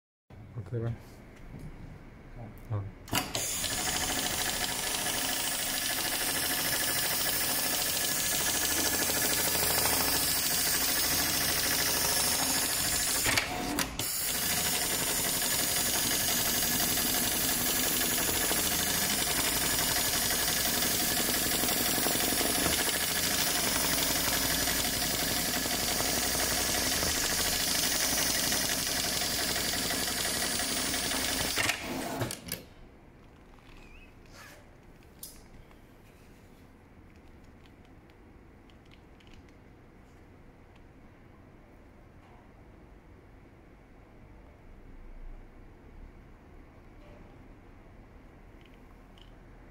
Programmable sewing machine for height safety harness Argentina
HighTex MLK500-2516 Most affordable automatic programmable sewing machine for manufacturer of height safety harness and fall protection equipment.
Deliver to Buenos Aires, Argentina